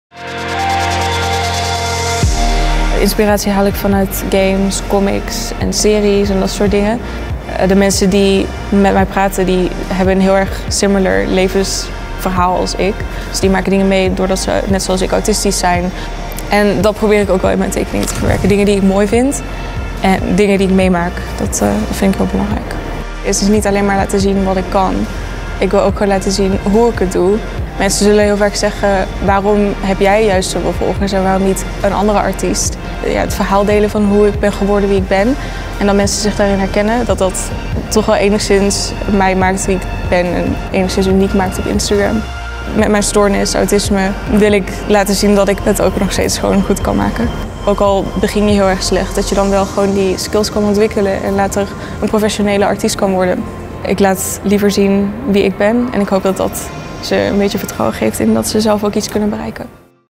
The Best Social Awards - Cyarine
In deze driedelige portretserie laten we samen met Huawei Mobile drie creatieve personen zien die impact maken met hun social kanalen en ons inspireren. Deze aflevering: Laura Brouwers (@cyarine).